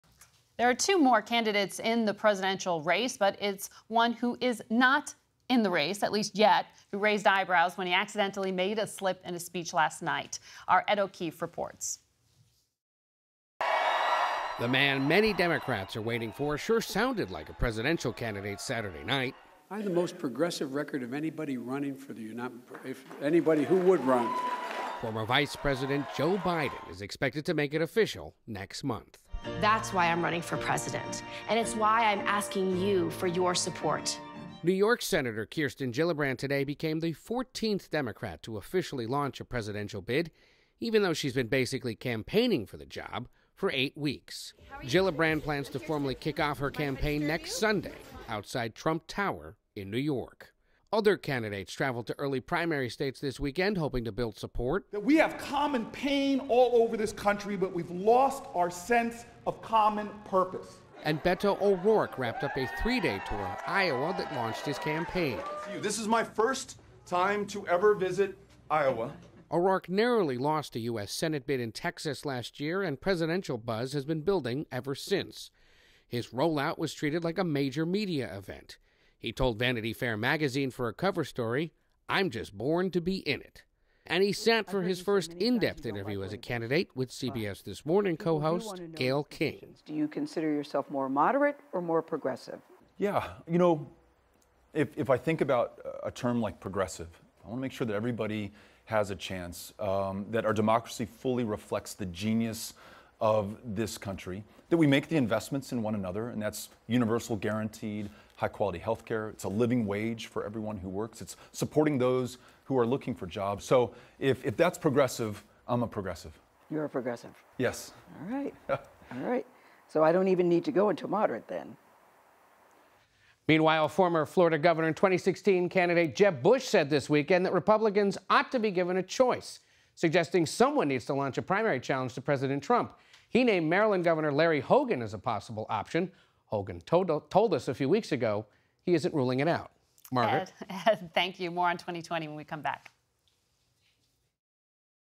As more Dems join the packed 2020 field, Biden teases his entry
CBS News political correspondent Ed O'Keefe reports on the official addition of Beto O'Rourke and Kirsten Gillibrand to the Democratic field, and Joe Biden's tease of running.